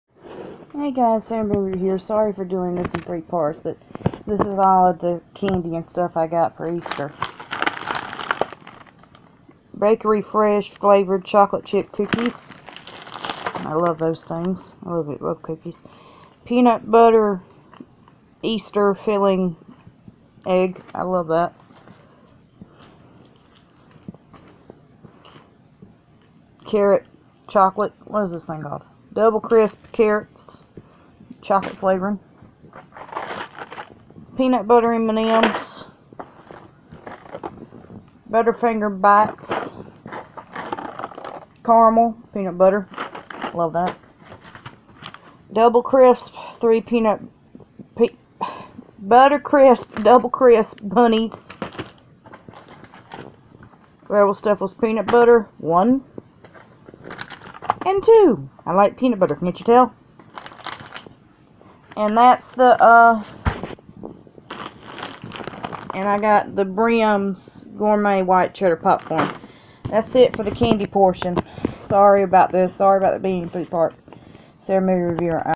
Part three of my Easter haul
The candy I got sorry that this is in three parts I haven't felt good for a while!